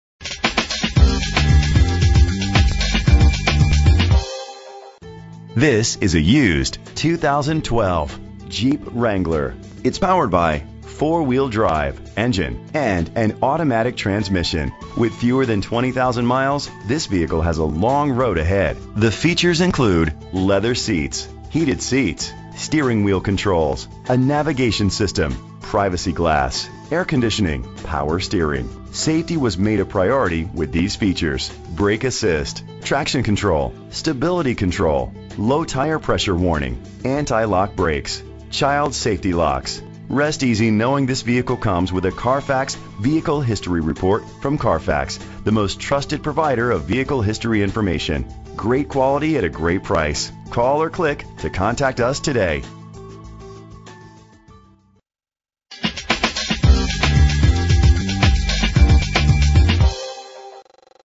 2012 Jeep Wrangler - Brighton MI
Year: 2012
Make: Jeep
Model: Wrangler
Trim: Rubicon
Engine: 3.6 L, 6 cylinder
Transmission:
Color: Red
Mileage: 15244
Address:
8240 West Grand River